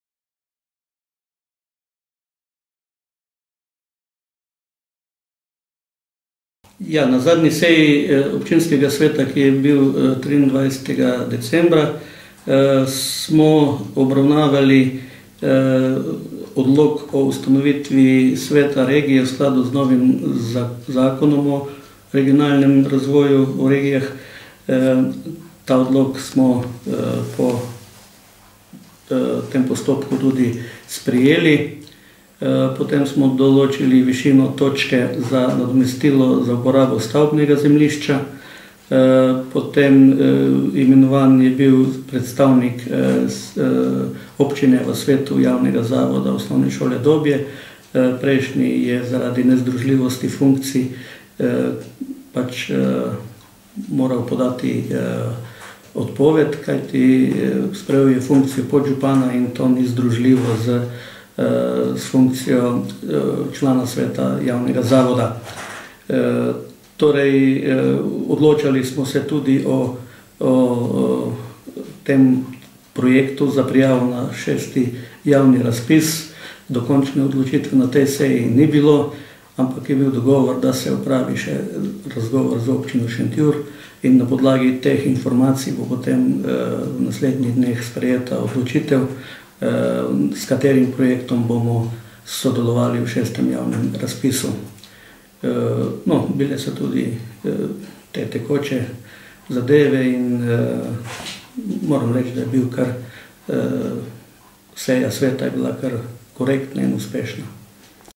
Franc Leskovšek o seji OS Dobje
Govori župan Občine Dobje